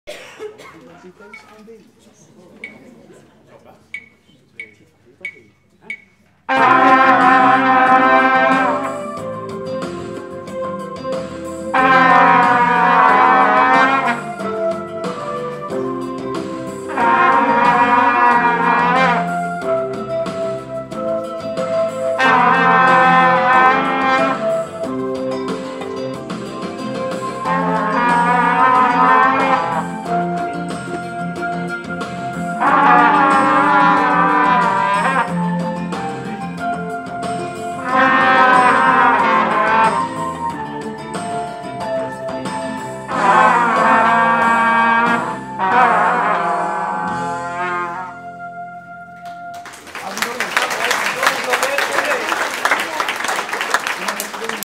Muziekkanjers - Trompetten liedje 1
Om leerlingen uit het basisonderwijs kennis te laten maken met blaasmuziek is er in samenwerking met Koninklijke Harmonie Unie Sevenum en Kunstencentrum Jerusalem een project gestart genaamd 'Muziekkkanjers'. Tijdens dit project zijn er muzieklessen verzorgd op diverse basisscholen in Sevenum.